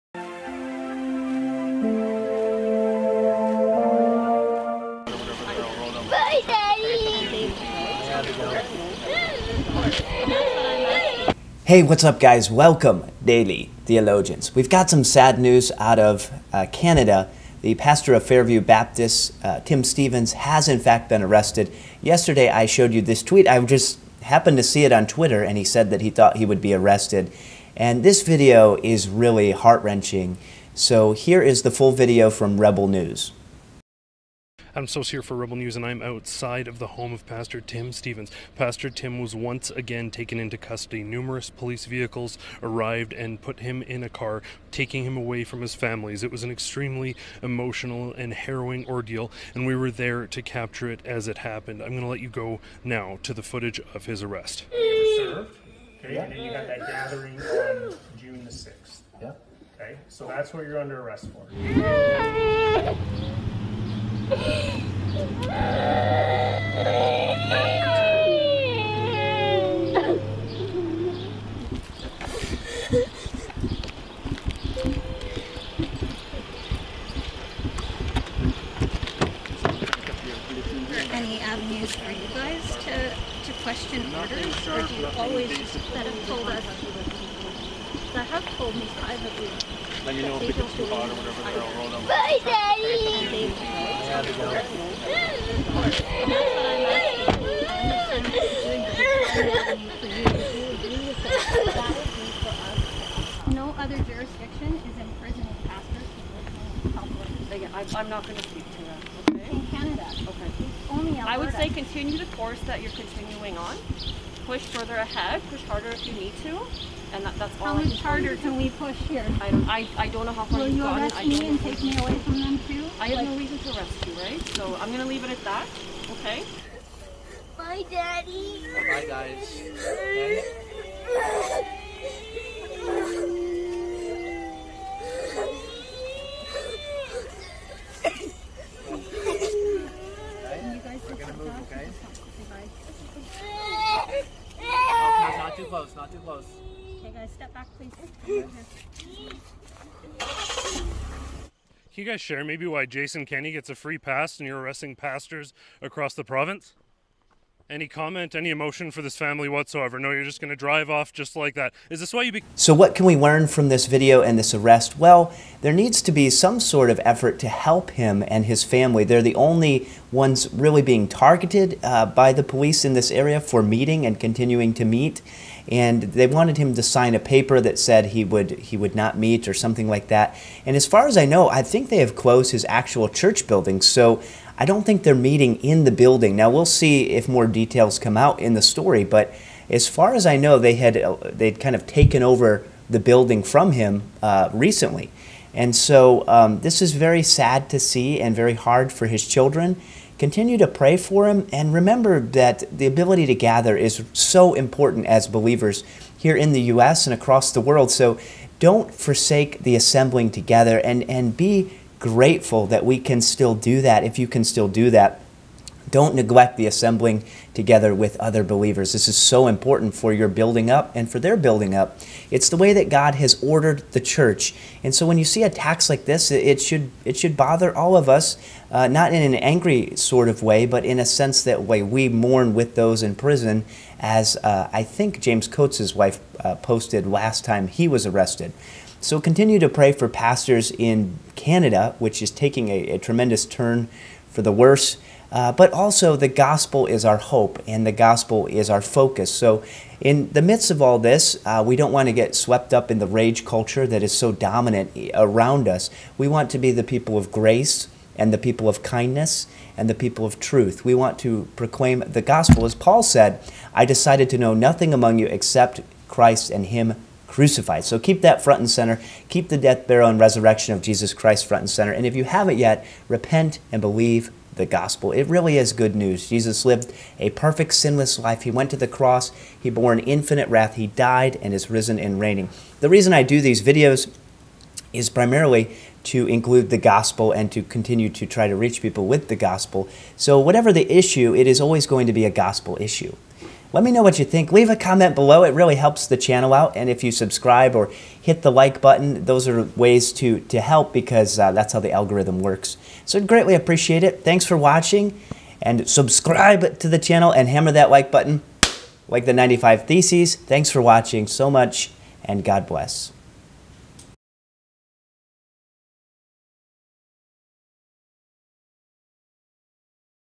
HEART WRENCHING SECOND ARREST of Pastor Tim Stephens
In this heart wrenching video watch the full clip of the 2nd arrest of Pastor Tim Stephens of Fairview Baptist Church in Canada.